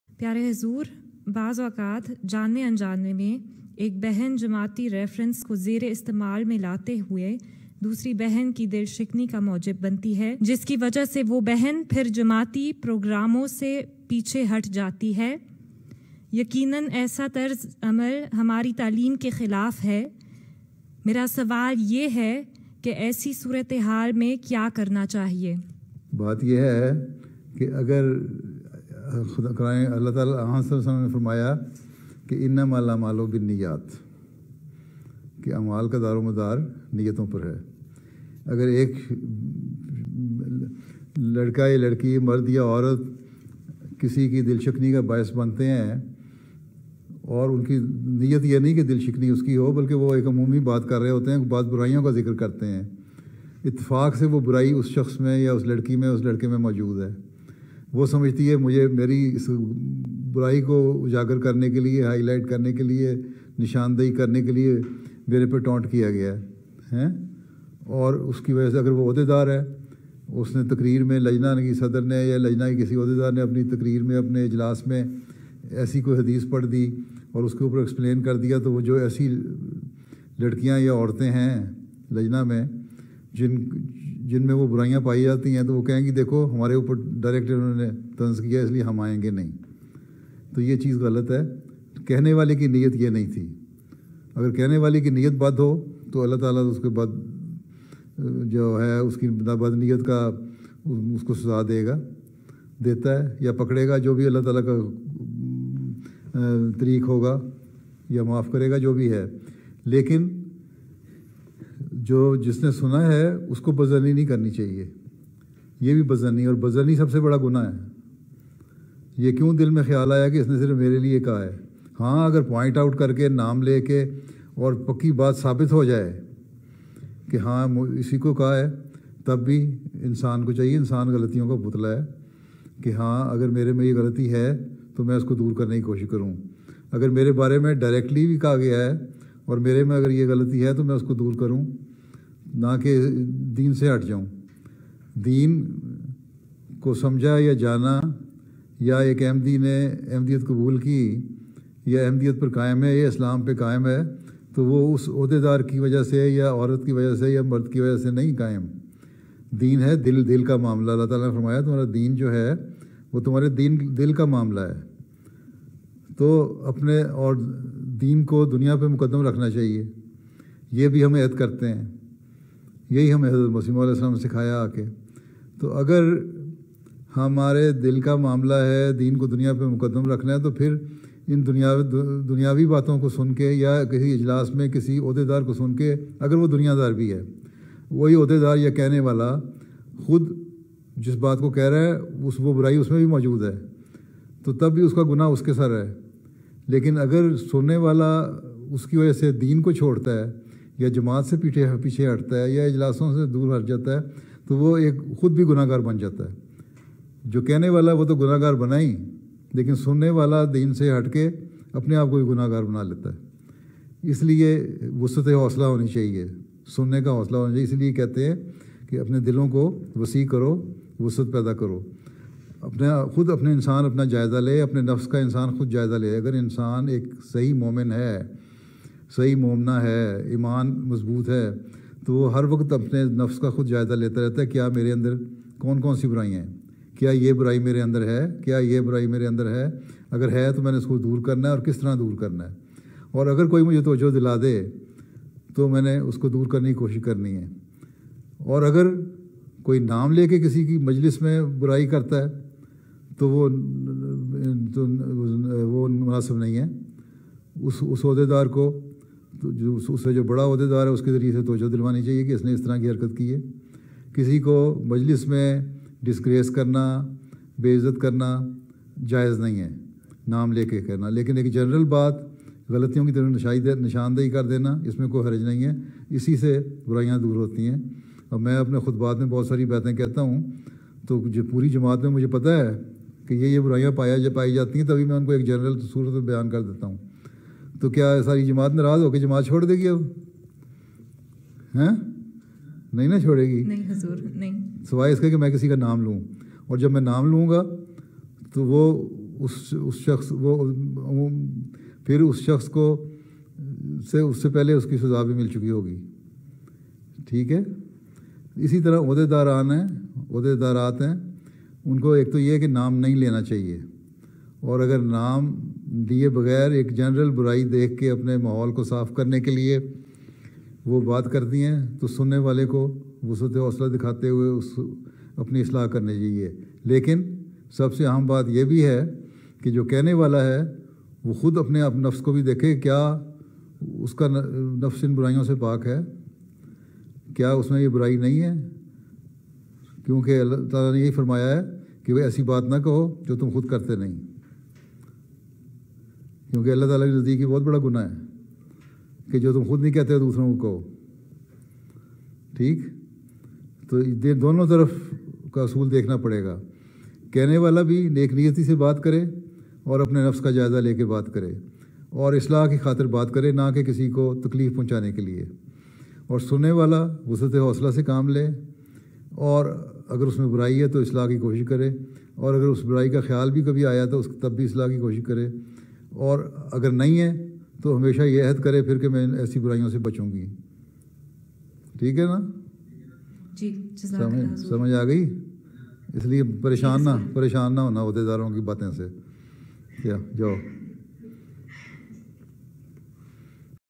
What to do when one sister is hurt by another sister & keeps herself away from Jamaat?
On 28 November 2021 Beloved Huzoor Hazrat Mirza Masroor Ahmad (May Allah be his Helper) took a class with Lazna Students of Germany.

On 21 November 2021 Beloved Huzoor Hazrat Mirza Masroor Ahmad (May Allah be his Helper) took a class with Sweden Lajna .